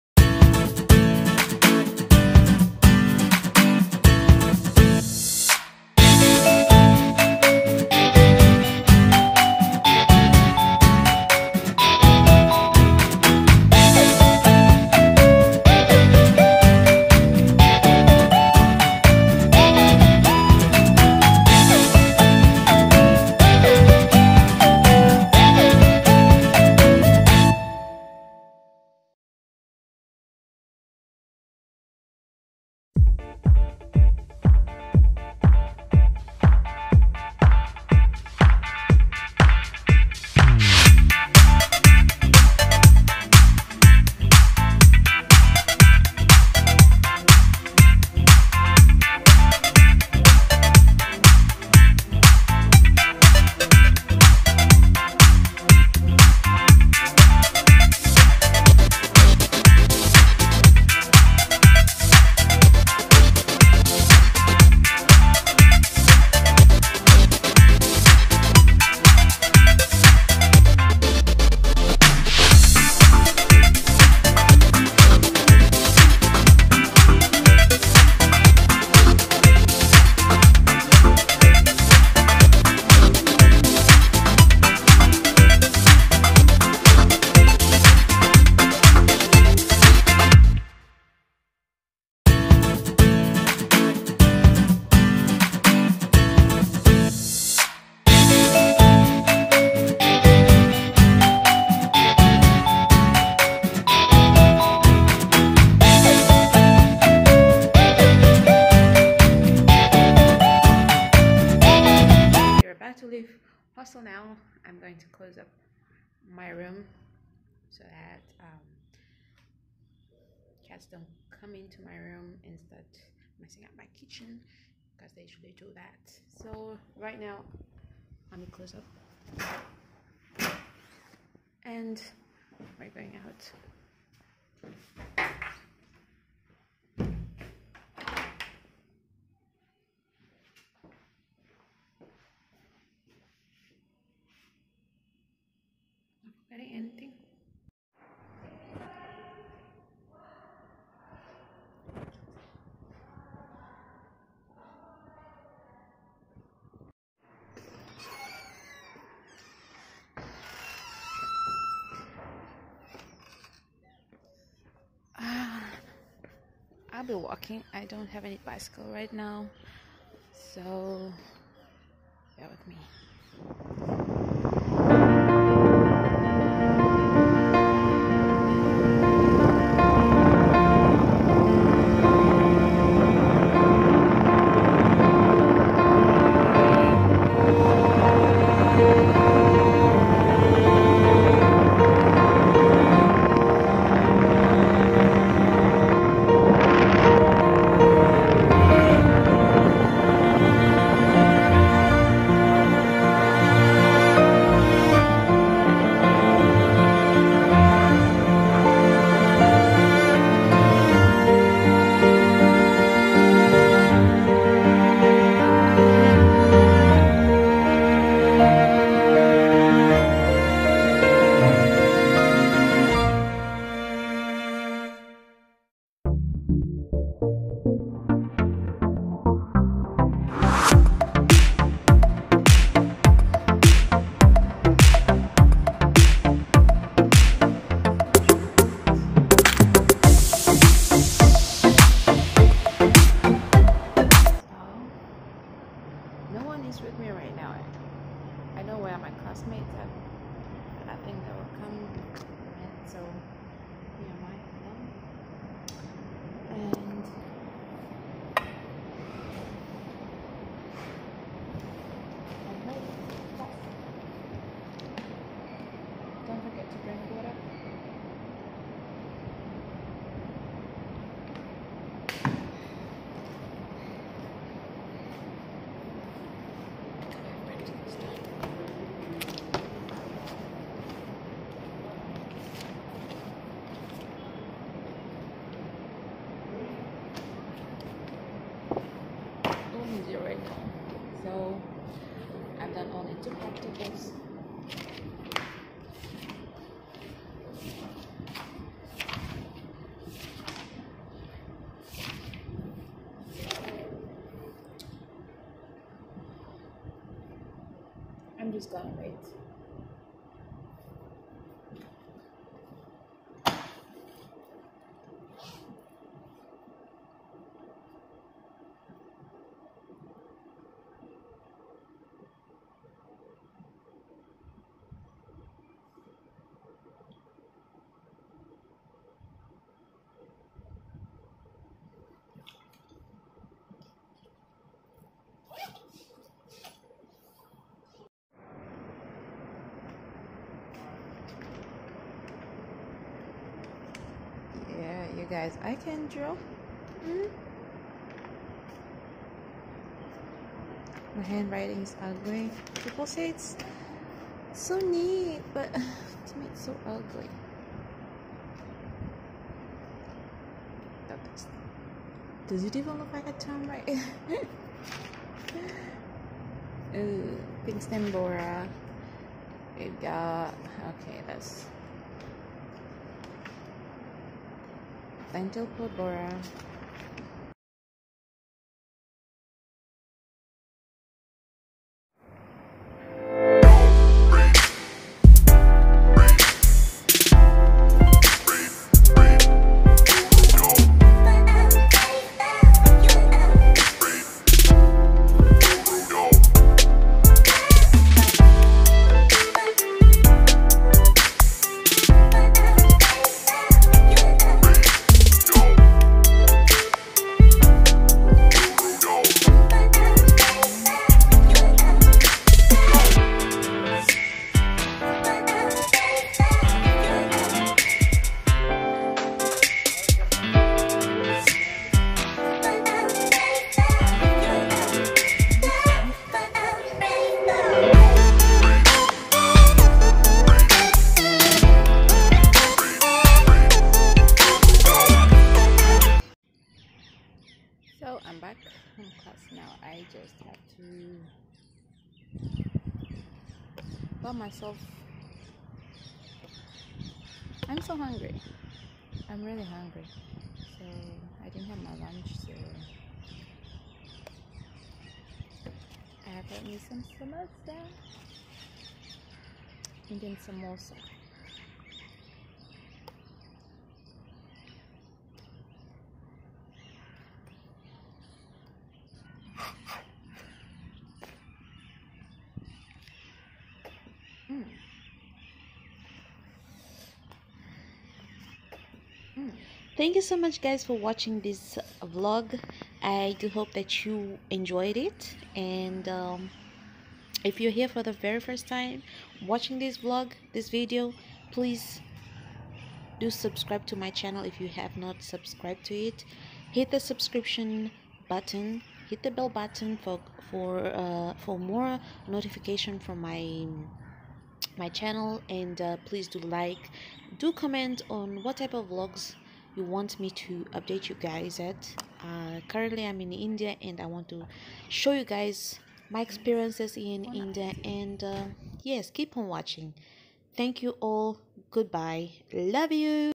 Student life in India
This video shows my everyday routine living as a student in India.
From hostel room to classes then back to hostel.
I hope you do enjoy this video😊